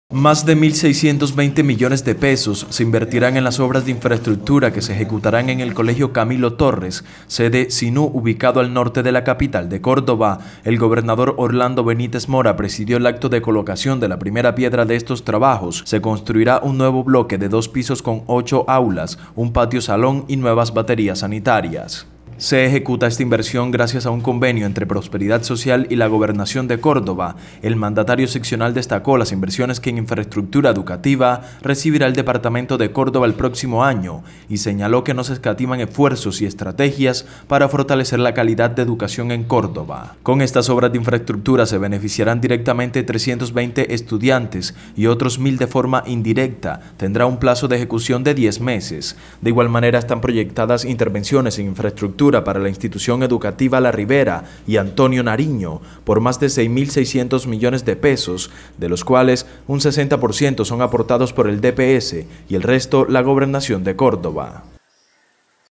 Más de $1.600 millones se invierten en nuevas aulas en I.E de Montería
Se ejecuta esta inversión gracias a un convenio entre Prosperidad Social y la Gobernación de Córdoba.

¡Siempre Informando! 📢